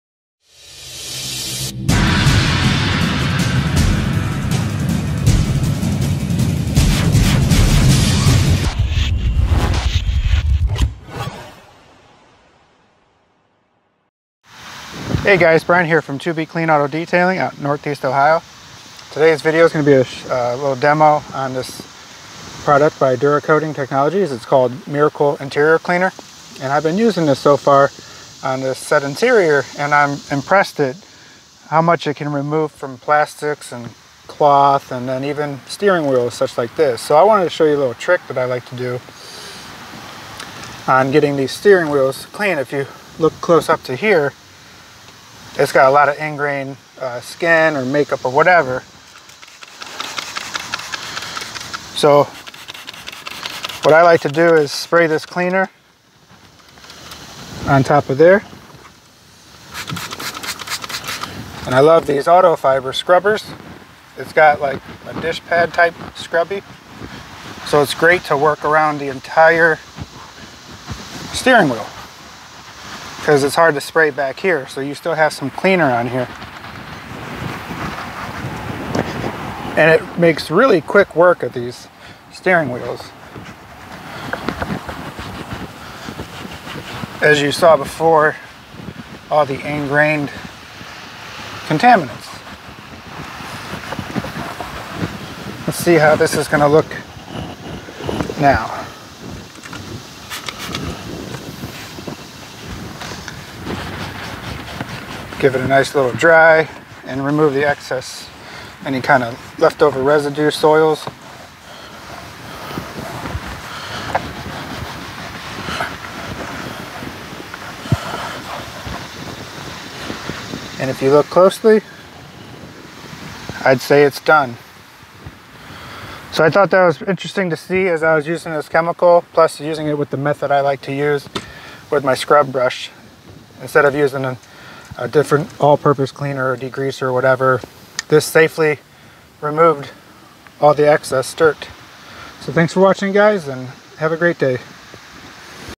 Fast and safe way to clean your steering wheel!
Steering wheels can be a pain to clean!  No need for brushes or microfibers just this simple pad.  Using a gentle and effective cleaner, coupled with an Autofiber Scrub Pad and your steering wheel woes are gone!!!

Make quick work of heavily soiled steering wheels with this scrub brush.  Super easy and effective!